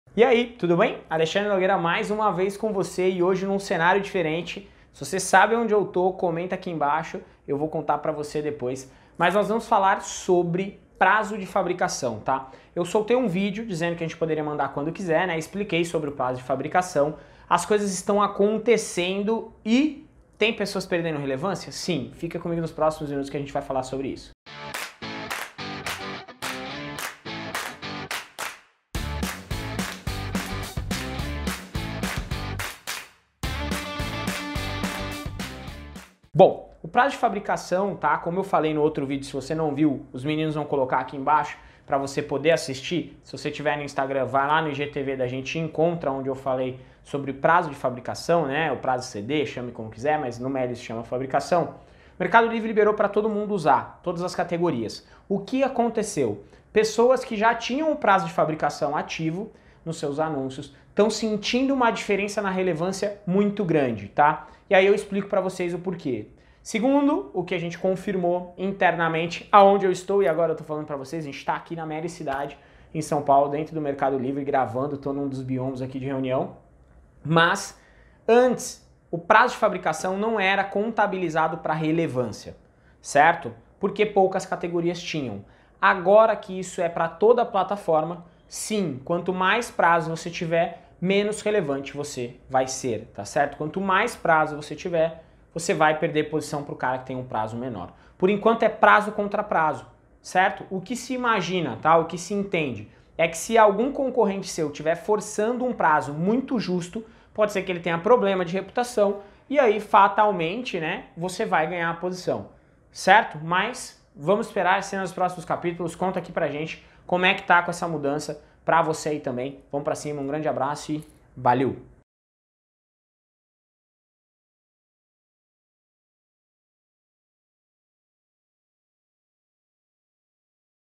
Prazo de Fabricação no Mercado Livre esta Tirando a Relevância dos Anúncios
As mudanças chegam, trazem perrengue consigo! O prazo CD foi liberado para todo mundo mas quem já usava anda no sufoco
Trouxemos essa notícia aqui no canal e cheguei a comentar numa suposição sobre o que poderia acontecer quanto aos níveis de relevância, e adivinhem só??
Confirmadíssimo.

Vendedores que já tinham prazos para fabricações dos seus produtos anteriormente a liberação para todos os setores, hoje precisam se adaptar aos novos rumos da plataforma e sambar para não perder visibilidade.
E foi diretamente da fonte, lá na Melicidade que entendi o porquê isso vem acontecendo.

Como tem sido essa nova fase, quais os problemas e como você achou soluções. Comenta ai com a gente!

 Bora pra cima, um grande abraço.